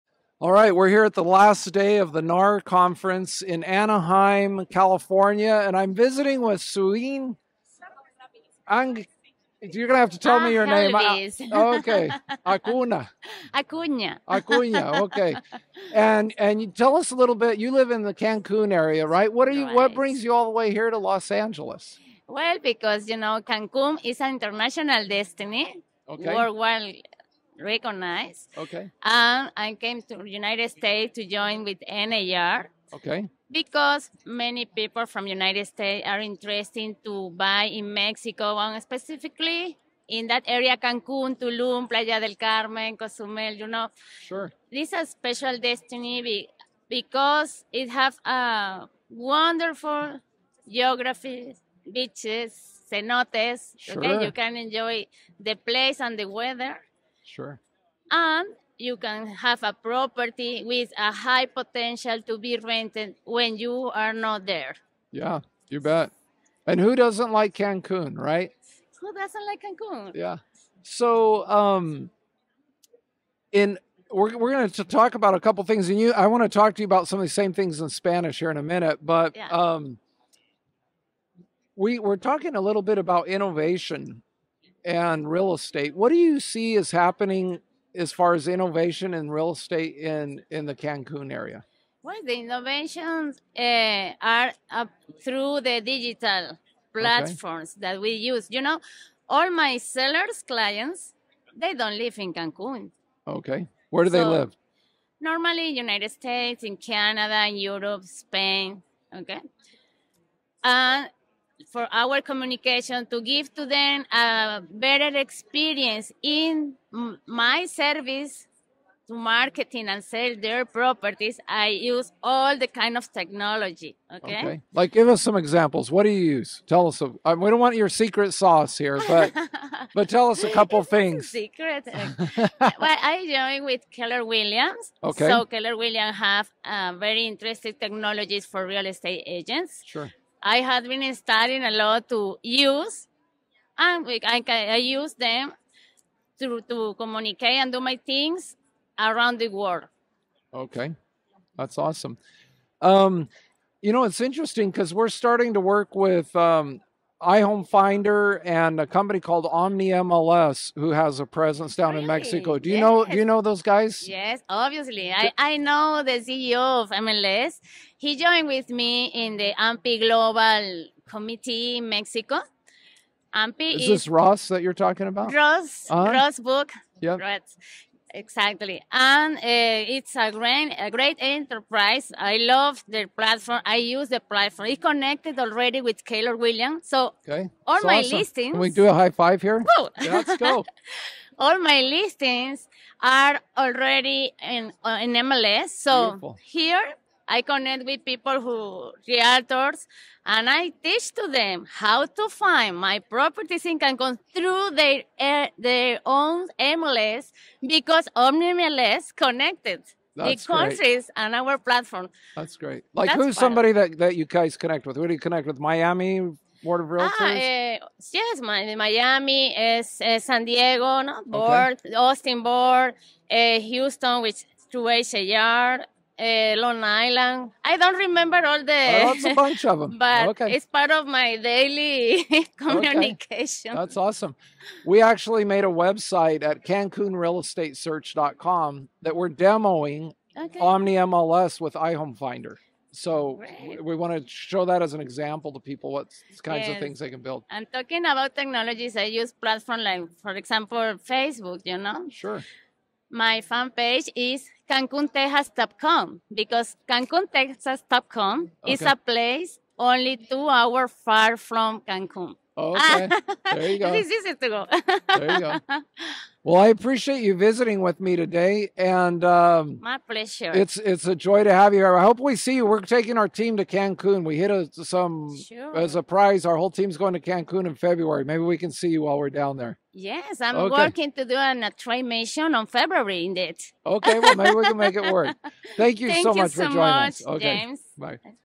Sueyin Agelvis English KW Keller Williams Realtor from Cancun Mexico at NARNXT discussing OmniMLS
Exploring Cancun's Real Estate with Sueyin Agelvis Acuña | NAR NXT Anaheim

In this insightful episode of our podcast, recorded live at NAR NXT in Anaheim, we had the pleasure of speaking with Sueyin Agelvis Acuña, a prominent figure from Keller Williams in Cancun, Mexico. Sueyin, a member of the Cancun MLS, shares her valuable perspectives on the burgeoning real estate market in Cancun and its appeal to international investors and homebuyers.

Key Highlights:

Cancun's International Appeal: Sueyin delves into why Cancun is increasingly becoming a hotspot for international real estate investment, emphasizing its beautiful landscapes, thriving tourism, and robust infrastructure.
The Role of Cancun MLS: She discusses the pivotal role of the Cancun MLS in streamlining real estate transactions and maintaining transparency in the market.
Collaboration with OmniMLS: A significant part of our conversation focuses on the innovative integration of OmniMLS. Sueyin explains how this platform allows for seamless sharing of real estate listings between Cancun and the United States, facilitating cross-border transactions and expanding market reach.
Market Trends and Opportunities: Sueyin provides insights into current market trends in Cancun, highlighting unique investment opportunities and offering advice for potential investors and homebuyers.
This episode is a must-listen for anyone interested in international real estate, particularly in the vibrant market of Cancun, Mexico. Sueyin Agelvis Acuña's expertise and insights offer a comprehensive understanding of the dynamics at play in this region's real estate sector.